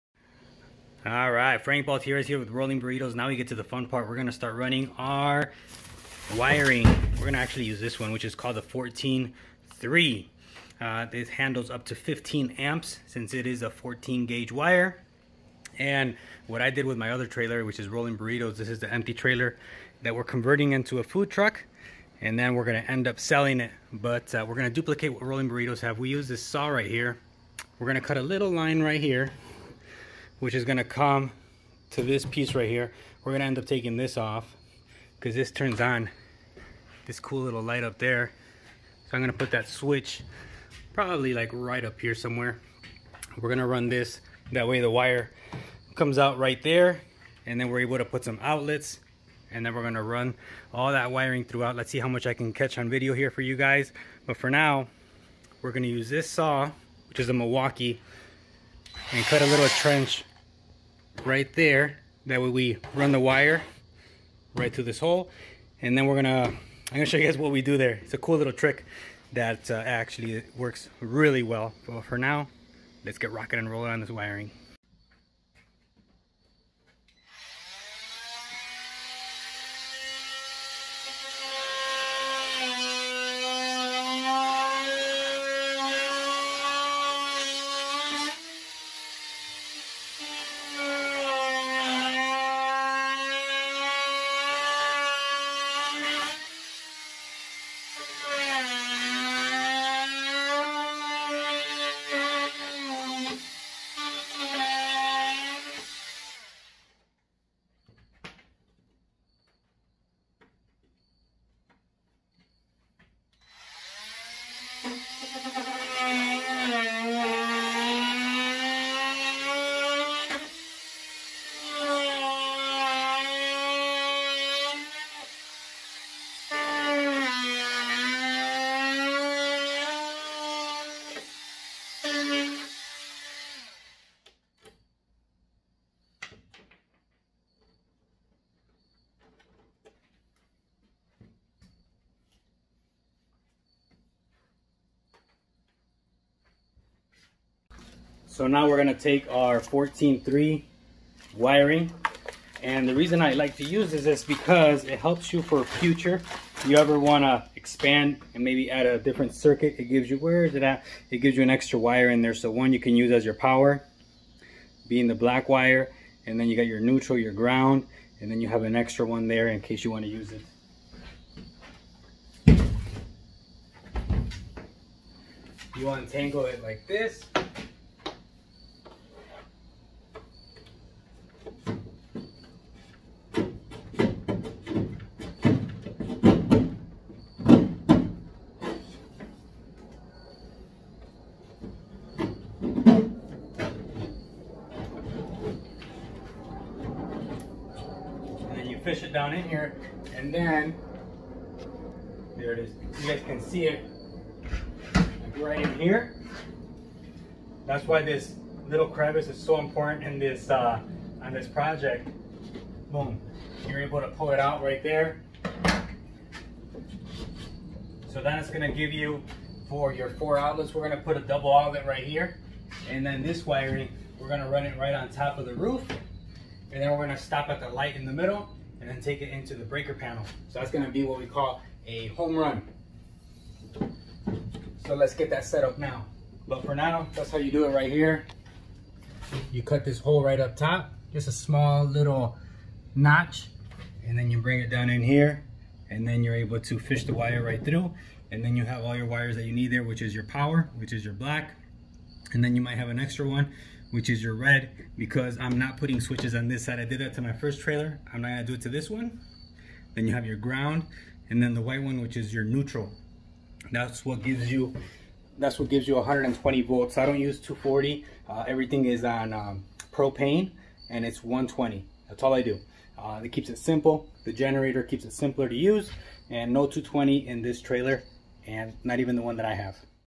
How to Build a Food Truck: Starting the Electrical
Don't try this at home, but if you do be careful. Watch out for sparks

any questions please do not hesitate to put them in comments section